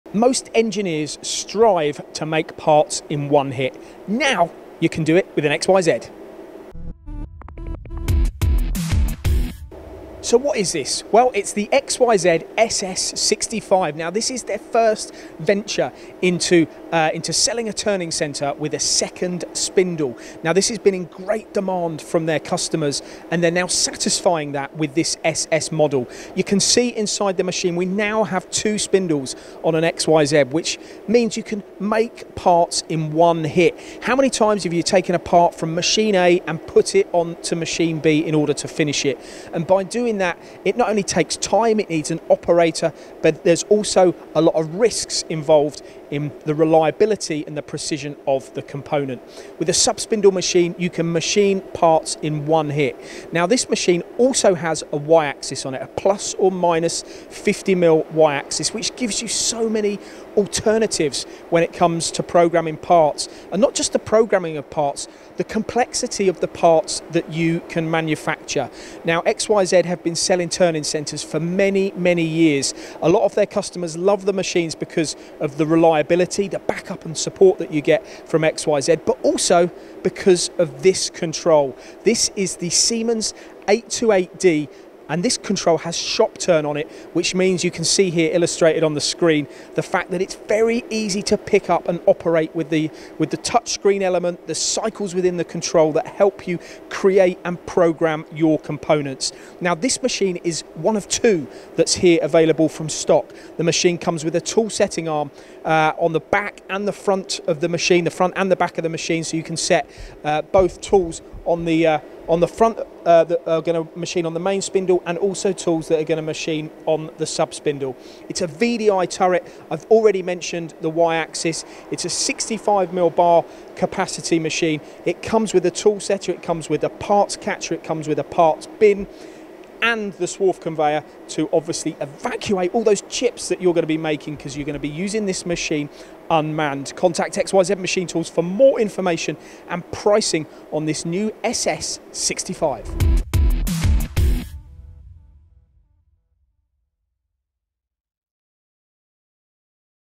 XYZ SS65 - Just Launched
Two spindles, one turret and a 100mm Y axis. The all new SS 65 turning centre, Paul takes a closer look and sees it in action.